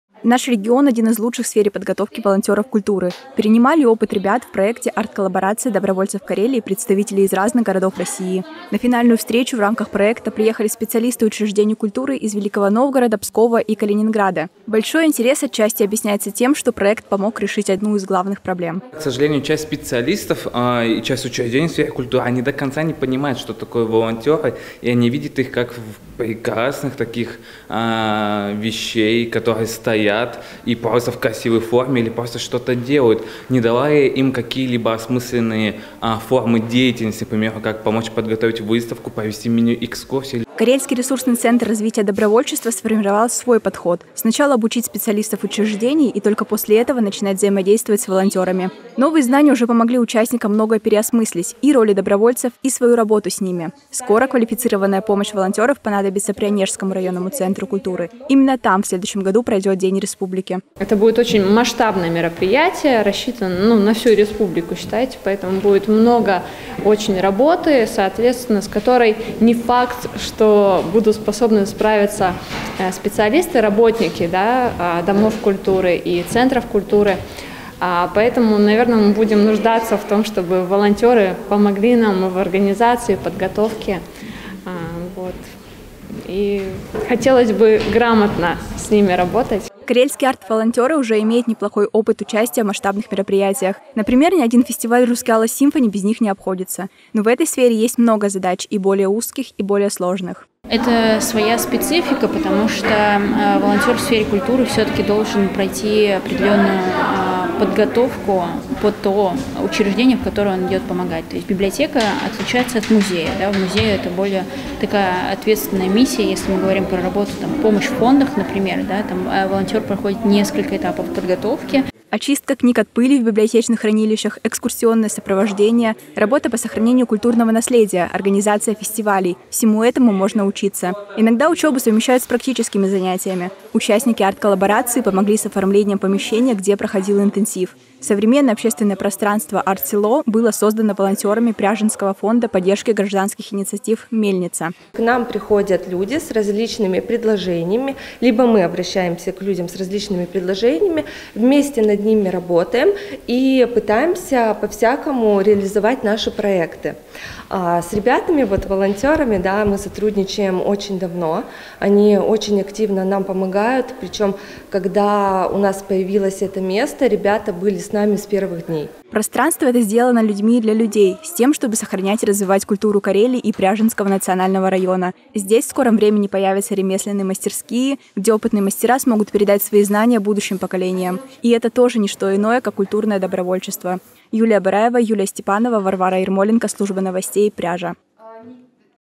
Проект «Арт-коллаборация добровольцев Карелии» завершает свою работу
В течение года волонтёры и специалисты сферы культуры учились взаимодействовать в деле сохранения наследия и создания новых форматов. Чему научились и кому будут помогать — узнаете в нашем сюжете.